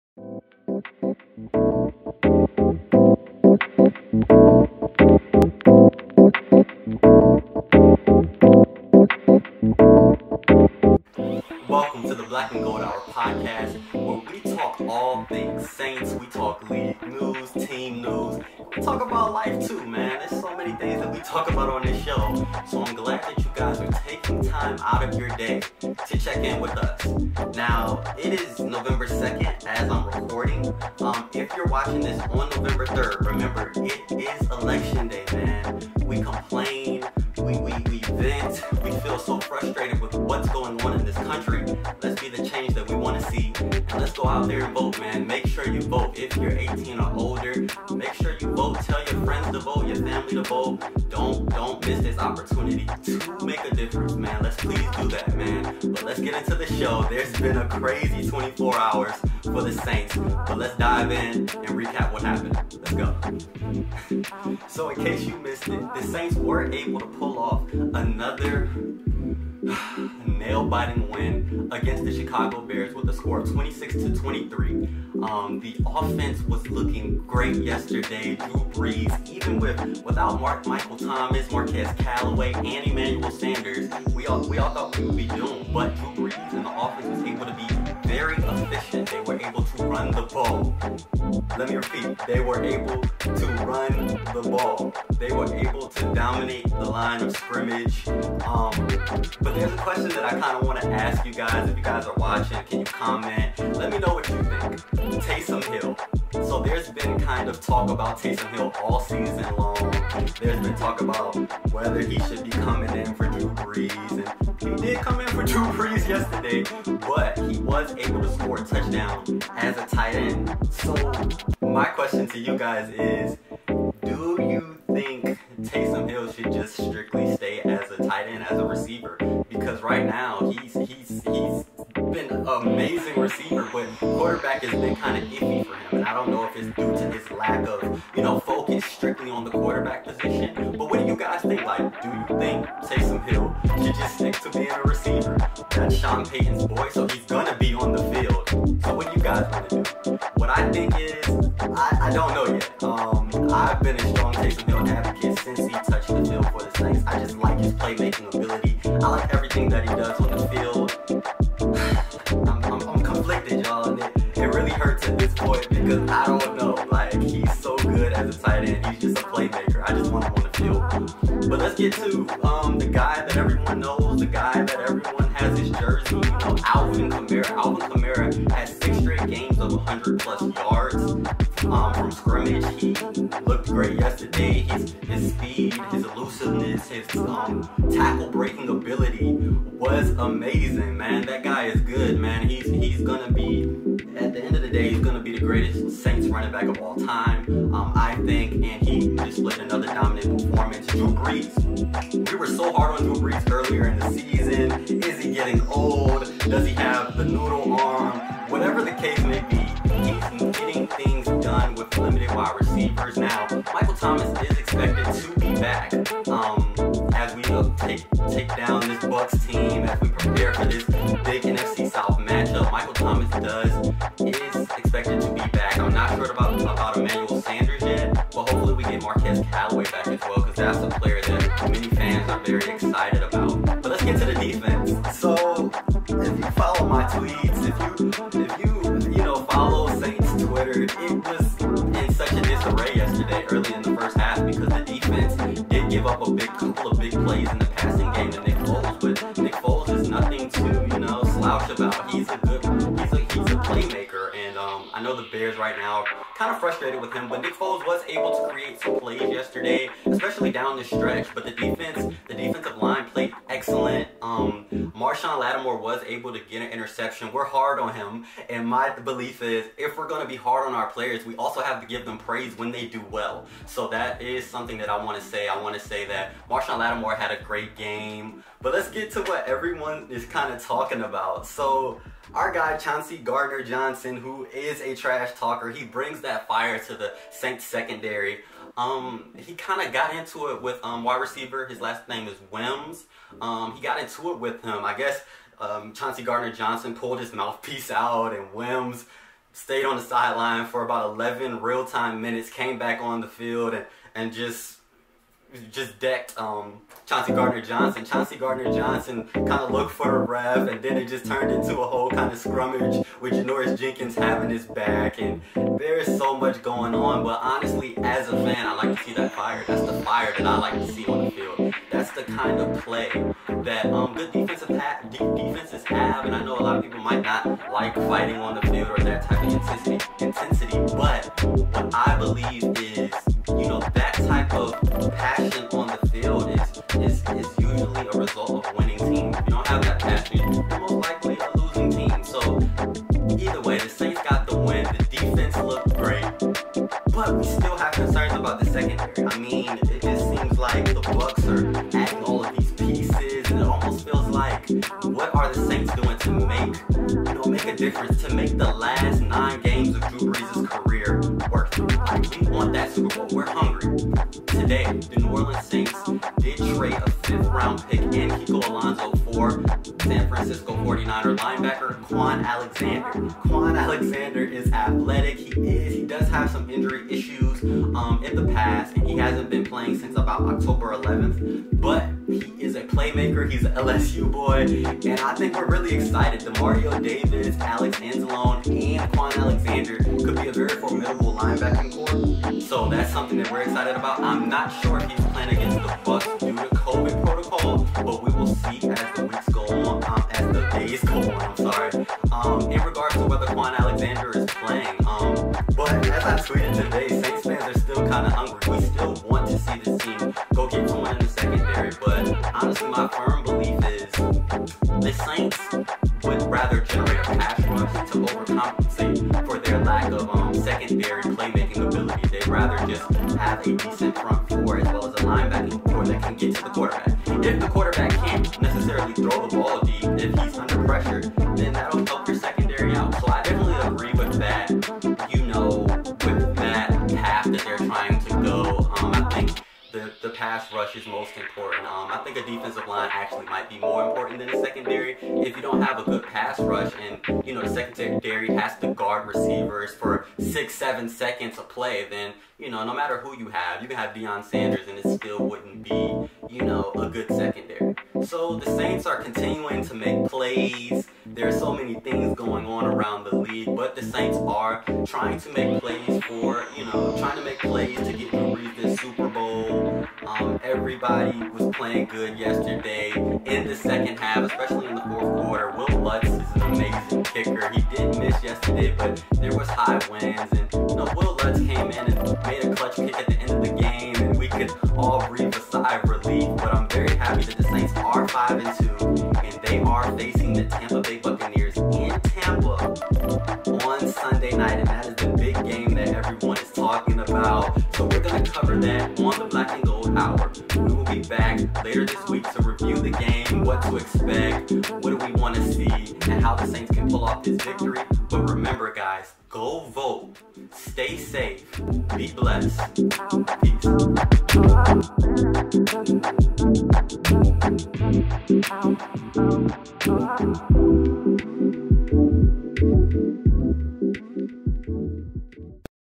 Black and Gold Hour Podcast Episode 114 ( Saints-Bears Post Game Review)
In this episode of the Black and Gold Hour podcast we will take a look at the Saints impressive road victory against the Chicago Bears. We will take a look at what players stood out, what things could be better, and how the Saints are continuing to win this season.

Be sure to VOTE. Be the change you want to see!